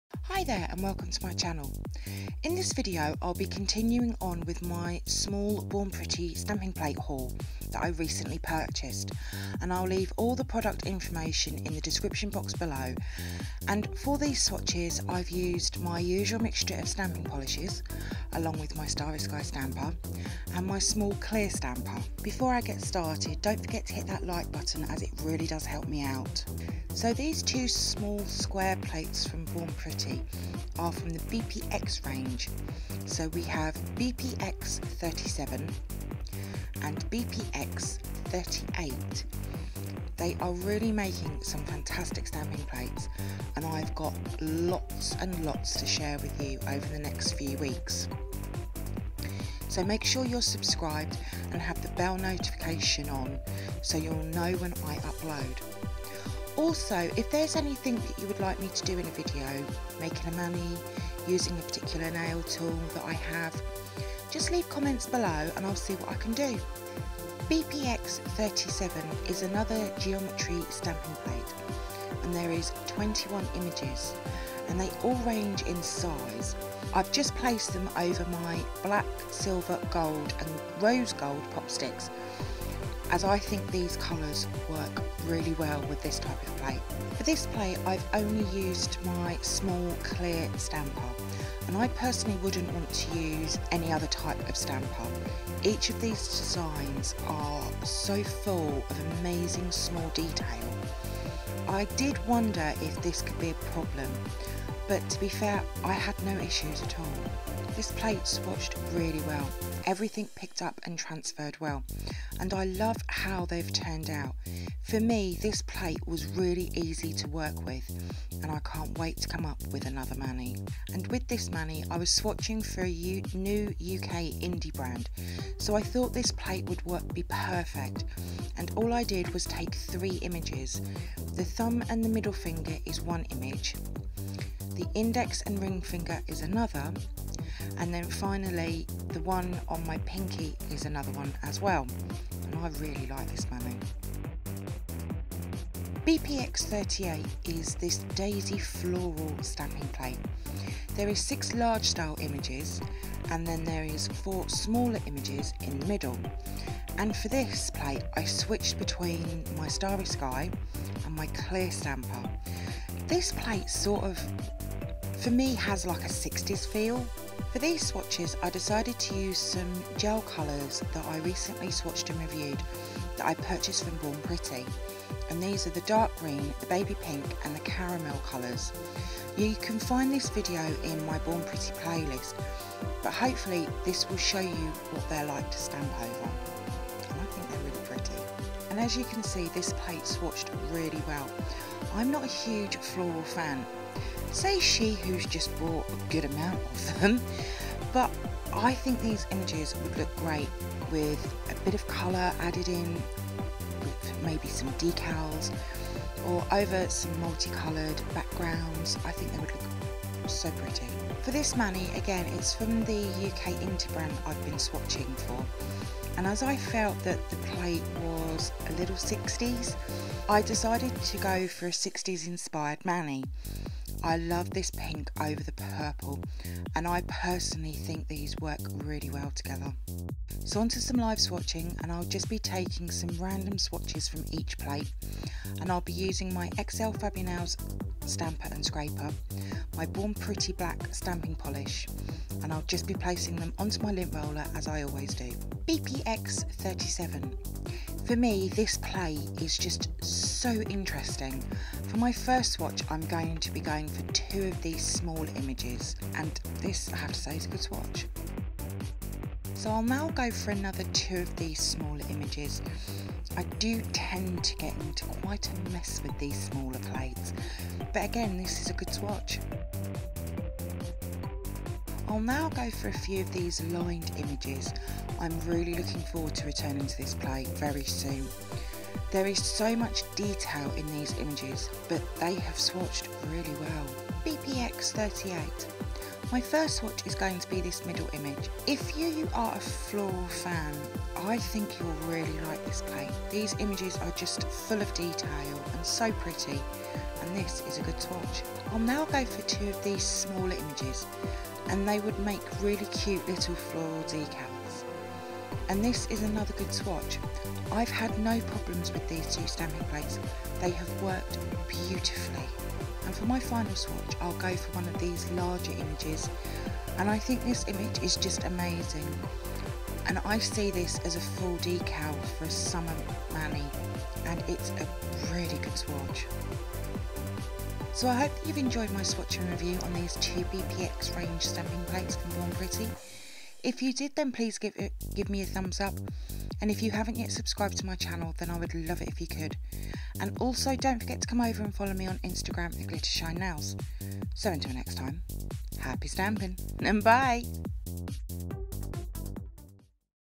Born Pretty Store Stamping Plates BPX-37 & BPX-38 Full Swatch And Review
Some more beautiful Born Pretty plates from the BP-X range. This time i have BP-X37 & BP-X38. Both stunning plates. I hope you like them.